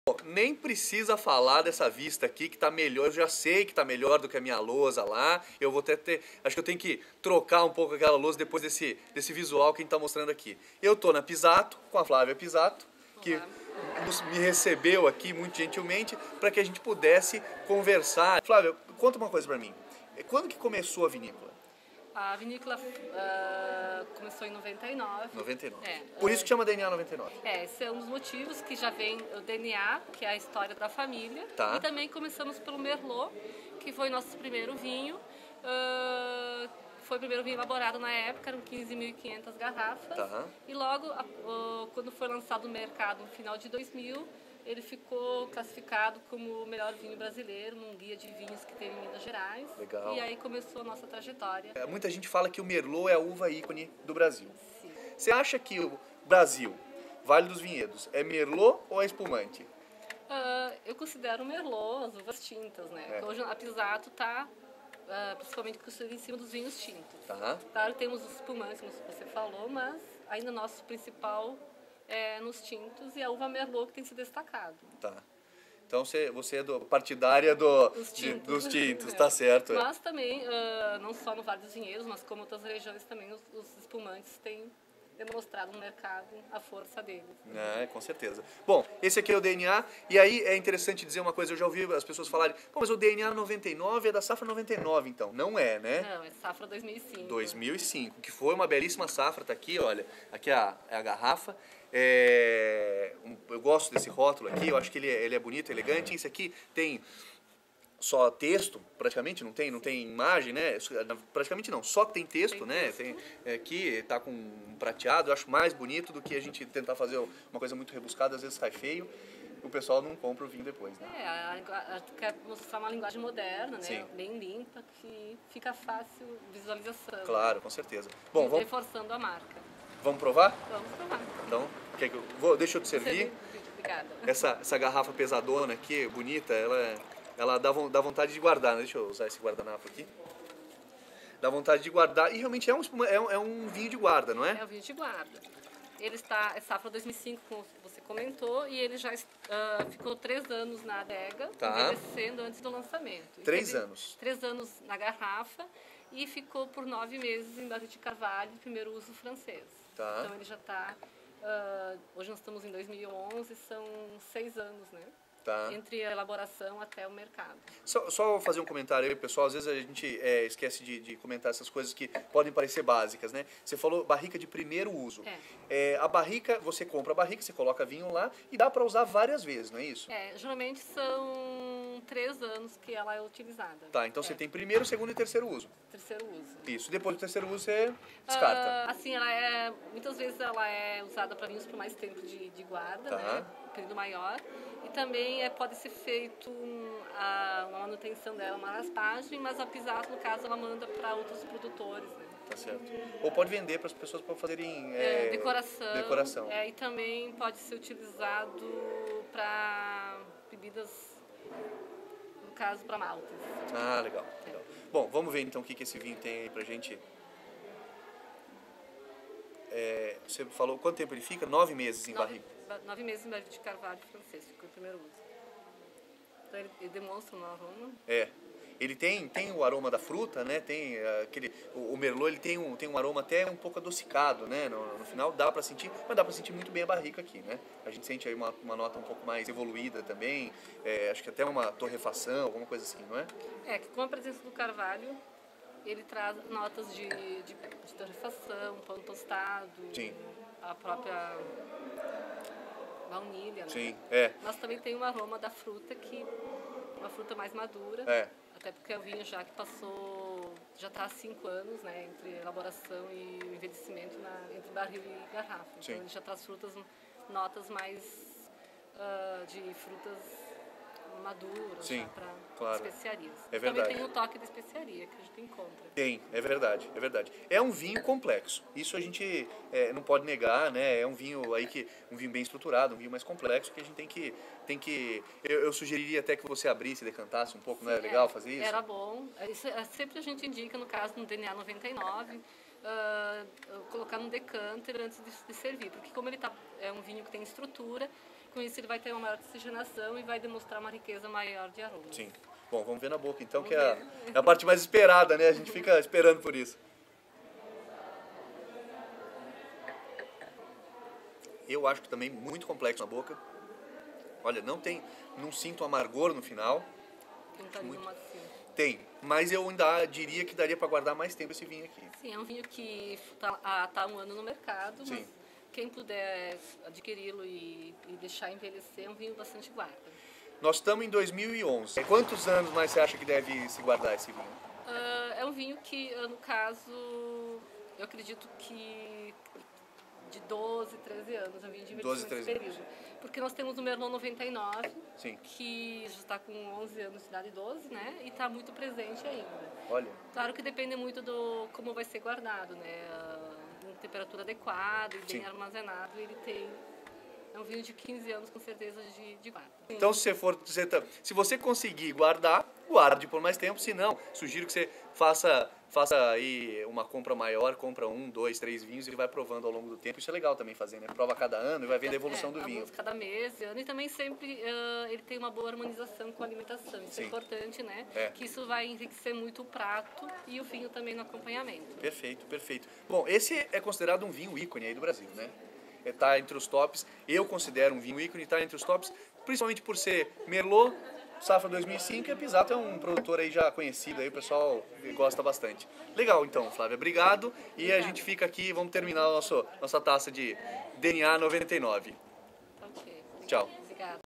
Pizzato DNA 99
Provei o Pizzato DNA 99 lá na vinícola, em companhia da Flavia Pizzato.